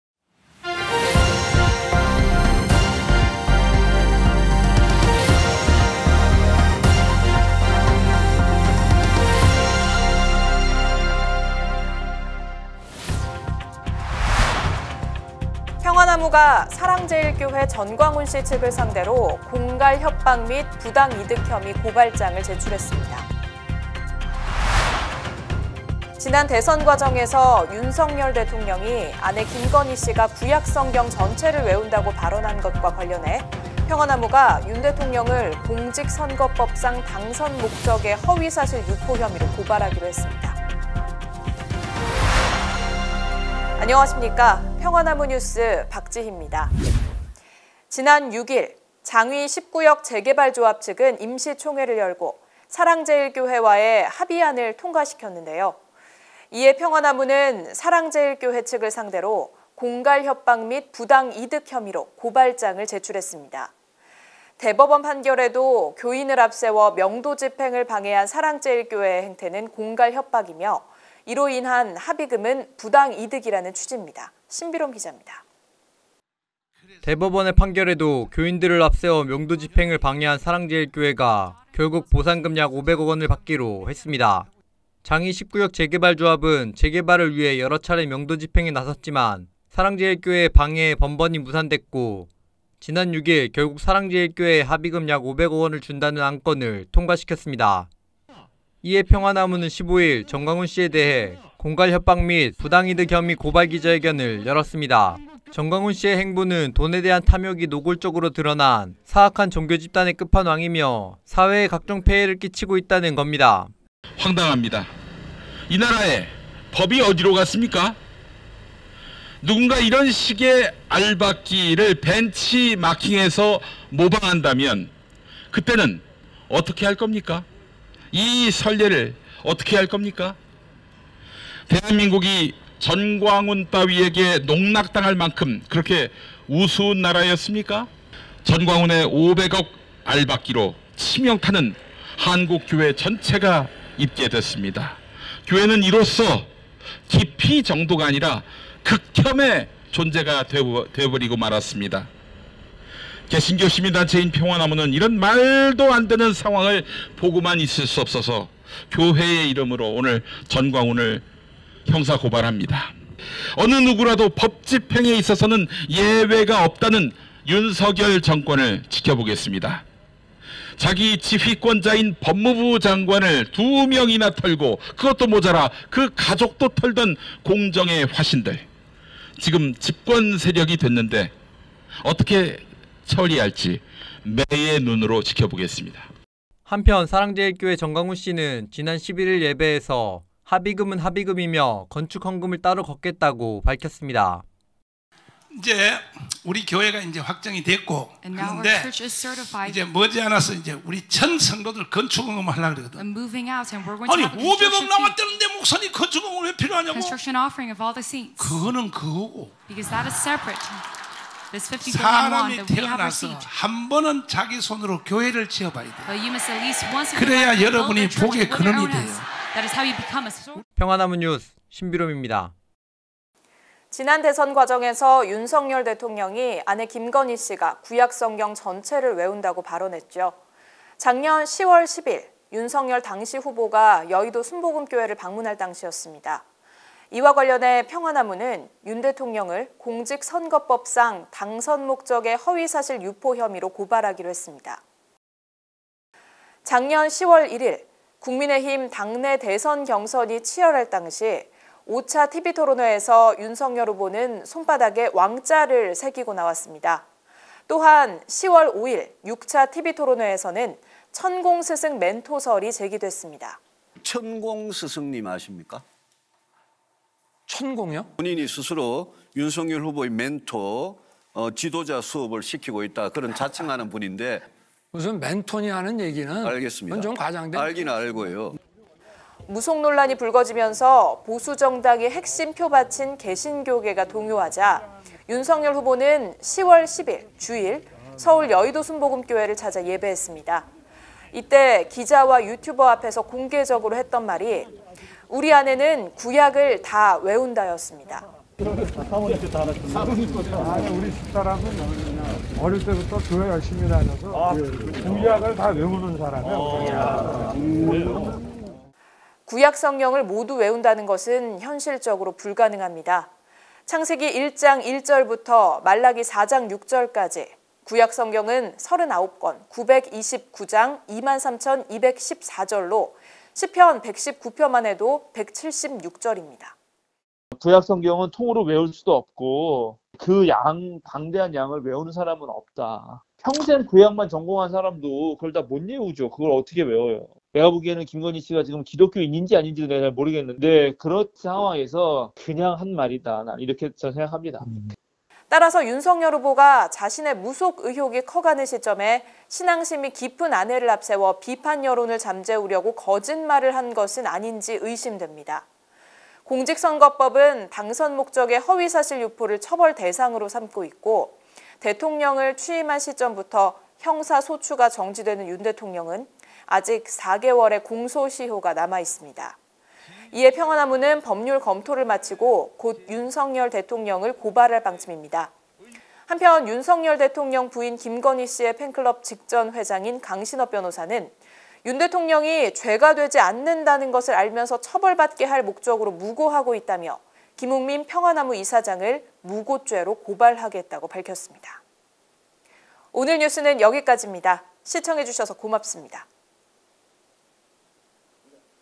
[평화나무뉴스 220915 전체영상] 평화나무 '알박기 전광훈' 고발!특수공갈 부당이득 혐의
평화나무 '알박기 전광훈' 고발!  특수공갈 부당이득 혐의

▪당신의 후원이 진실된 보도를 만들고 바른 교회를 세웁니다
   [일시후원 계좌번호]
    하나은행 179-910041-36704 (사단법인 평화나무)